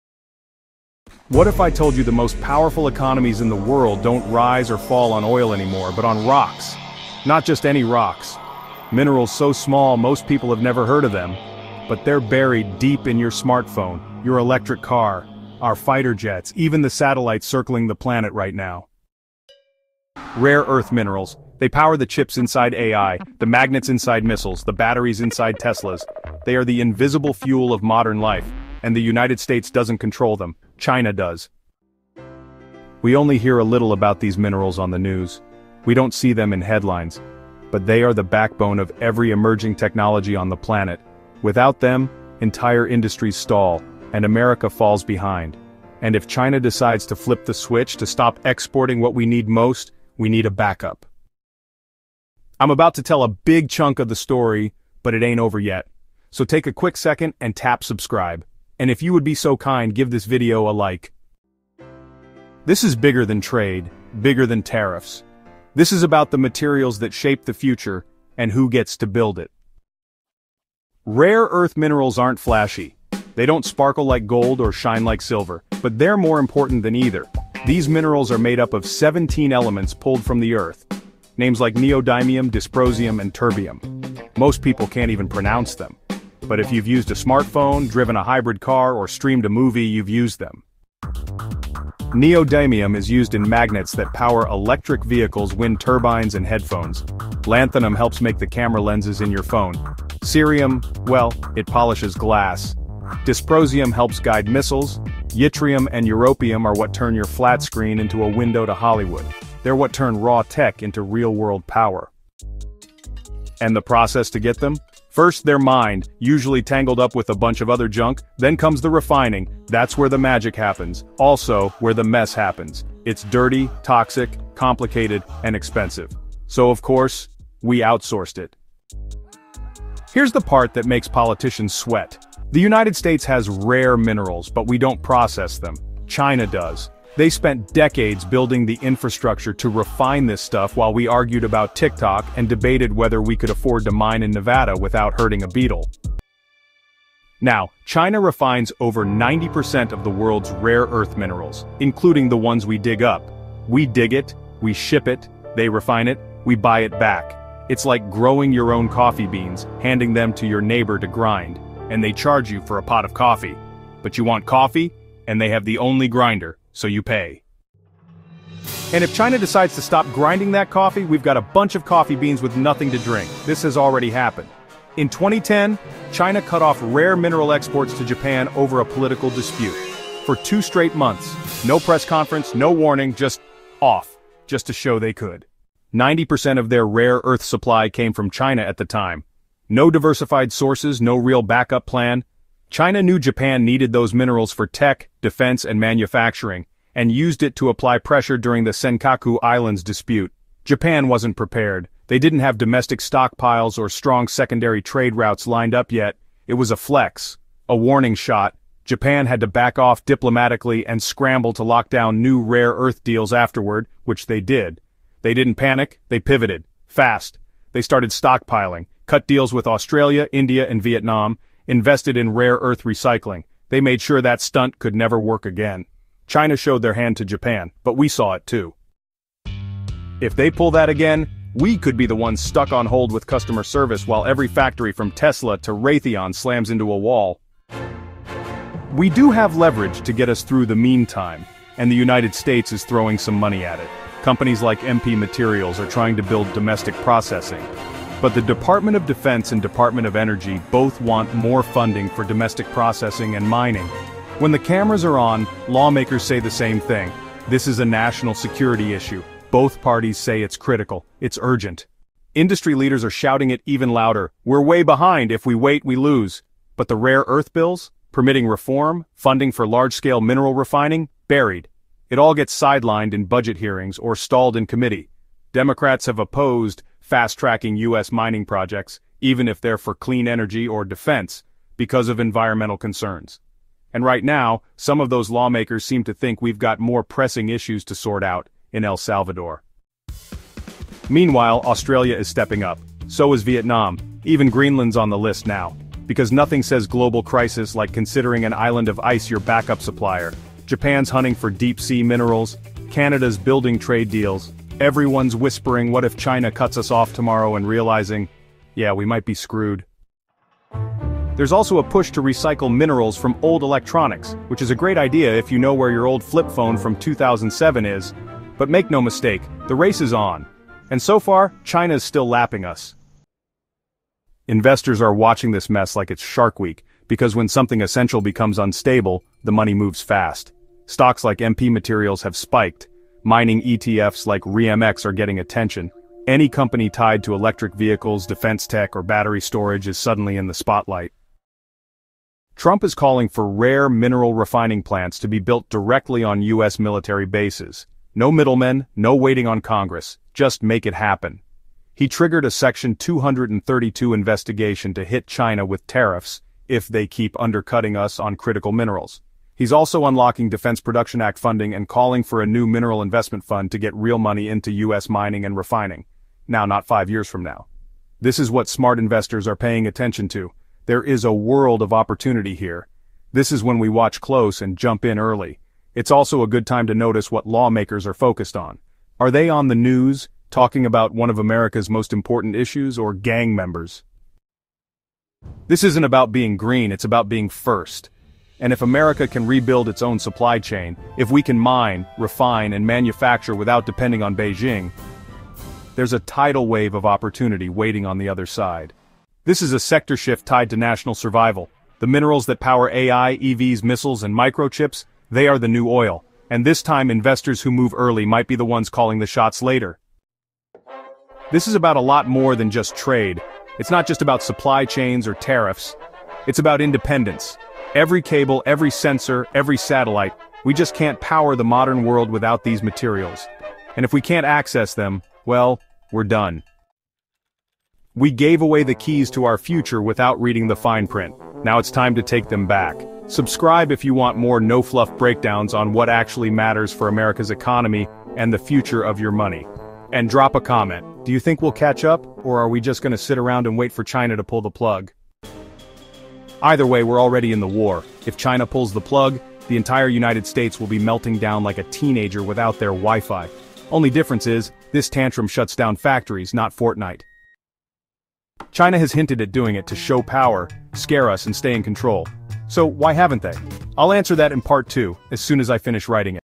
China Holds the Chips. But Who Holds the Cards? Mineral Warfare
They power our phones. Our jets. Our future.
And China controls the supply.

Rare earth minerals aren’t rare anymore—they’re essential.
We can’t build without them. We can’t fight without them.
And right now, the United States is dangerously dependent on the one country threatening to pull the plug.

This isn’t just about trade.
It’s about power, strategy, and survival in a tech-driven world.

If you care about America’s future, economic freedom, or your own investments—watch this all the way through.
Because if China flips the switch, we don’t just lose chips…
We lose control.

👇 COMMENT: Do you think we can catch up—or is it already too late?
👍 Like if you want more no-fluff breakdowns.

OUTLINE:
00:00:00
The Rare Mineral Crisis

00:00:19
The Invisible Fuel

00:00:31
The Backbone of Emerging Technology

00:00:51
Untitled Chapter

00:01:01
The Future of Materials

00:01:09
What They Are, What We Use

00:01:30
Uses of Rare Earth Minerals

00:01:52
The Outsourcing Problem

00:02:08
The China Chokehold

00:02:26
China's Dominance

00:02:47
The Risk of China Cutting Supply

00:03:05
Untitled Chapter

00:03:48
Consequences of Supply Interruption

00:03:57
The Global Scramble

00:04:22
Untitled Chapter

00:04:49
Other Countries Stepping Up

00:05:10
Recycling and the Ongoing Race

00:05:23
What It Means For Investors

00:05:41
Untitled Chapter

00:06:24
The Opportunity

00:06:39
A Sector Shift

00:06:50
The Core Issue

00:07:06
Losing Control

00:07:25
The Stakes

00:07:37
Teaser for Part Two